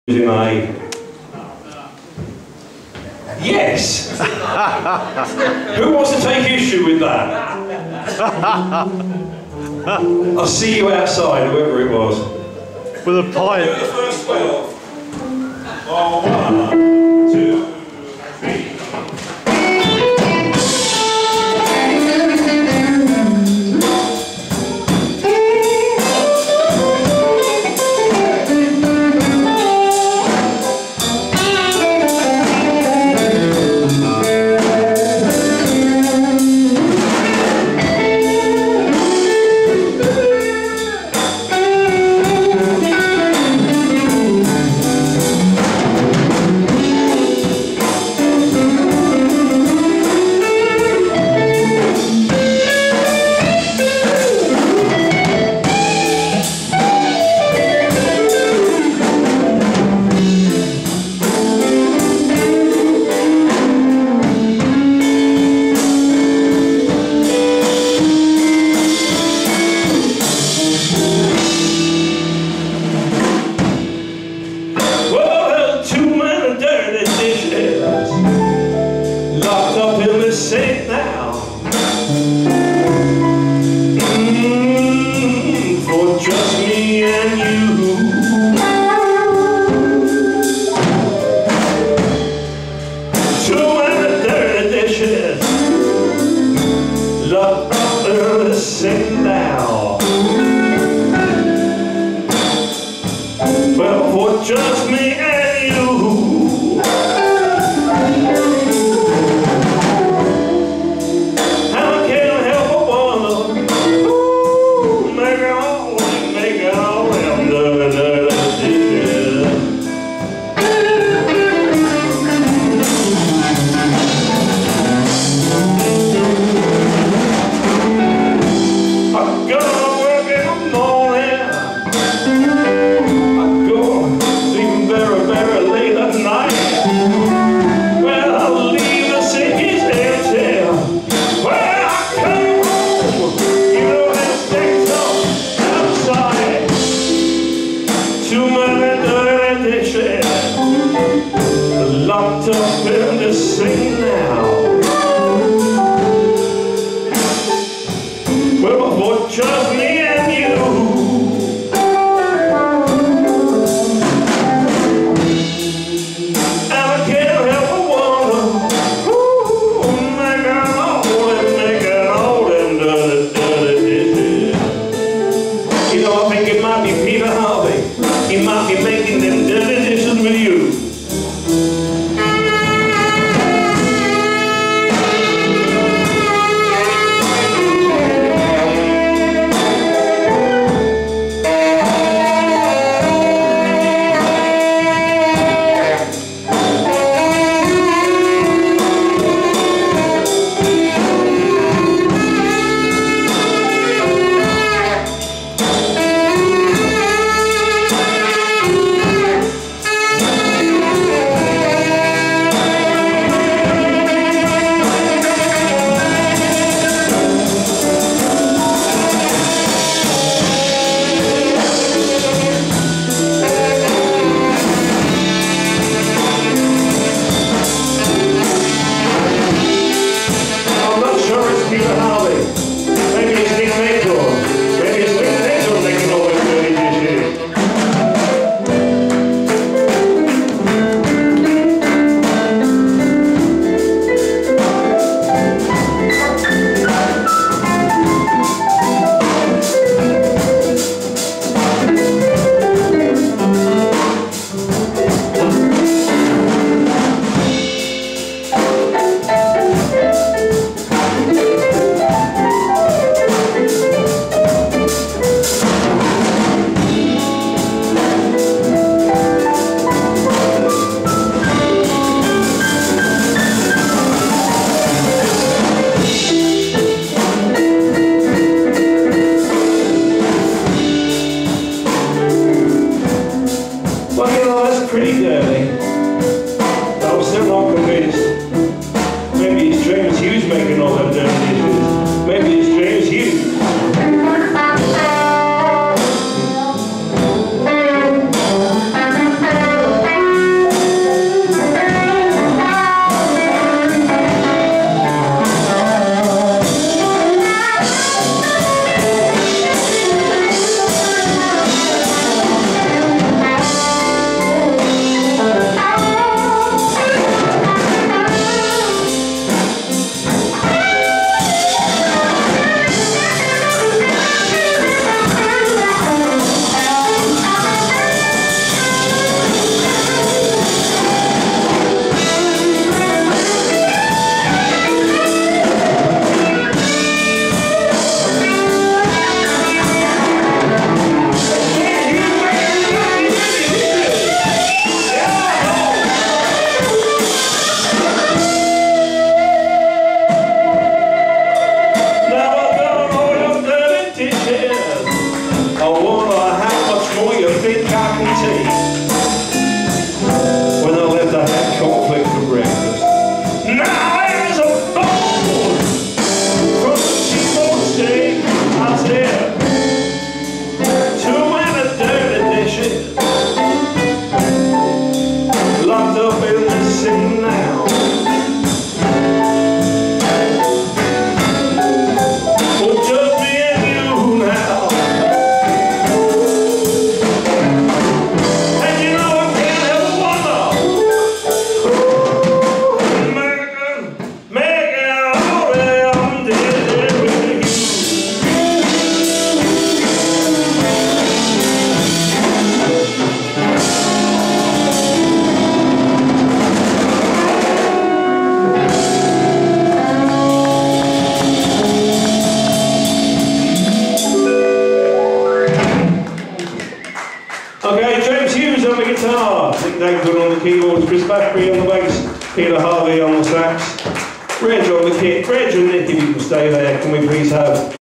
BLUES AT THE COACH AND HORSES 10 7 13
COACHANDHORSESBLUESJAM.COM
STEVIE KING   GUITAR VOC
PETER HARVIE   SAX
NICK NAKORN   KEYBOARD
JAMES HUGHES    GUITAR